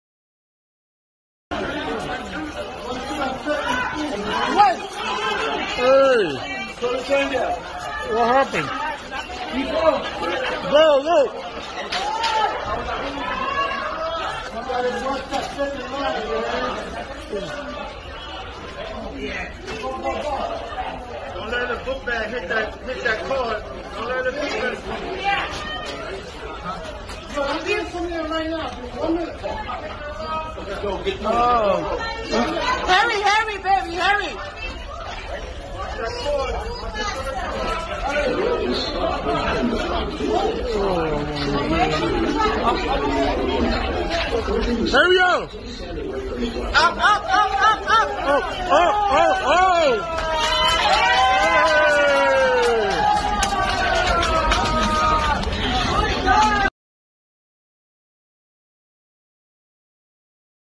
Rescatan a hombre de ser arollado por tren en Nueva York
Una persona sufrió un episodio médico cuando se encontraba en una de las estaciones subterráneas del metro de Nueva York, pero afortunadamente dos oficiales se encontraban en el mismo lugar.